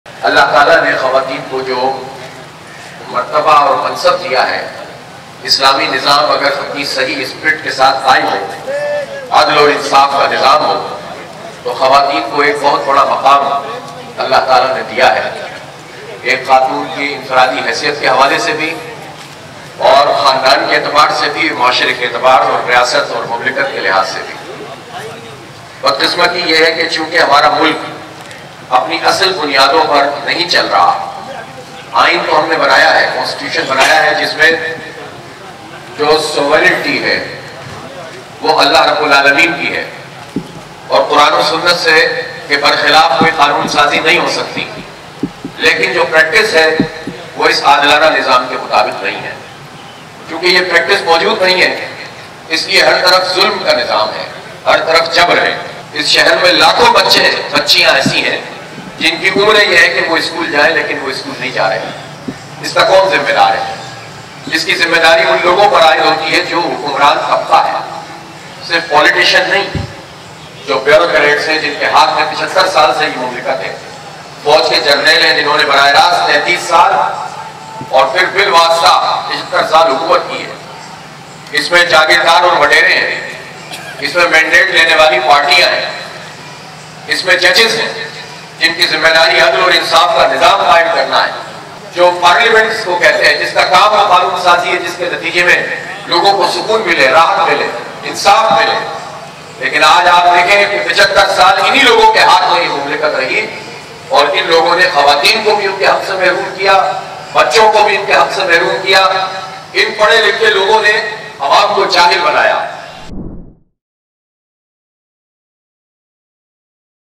ہمارے حکمران طبقے نے 75 سال سے خواتین کو اُن کے حقوق سے محروم رکھا ہوا ہے۔ حافظ نعیم الرحمن
اللہ تعالیٰ نے خواتین کو بہترین مقام اور منصب دیا ہے لیکن ہمارے حکمران طبقے نے 75 سال سے خواتین کو اُن کے حقوق سے محروم رکھا ہوا ہے۔ حافظ نعیم الرحمن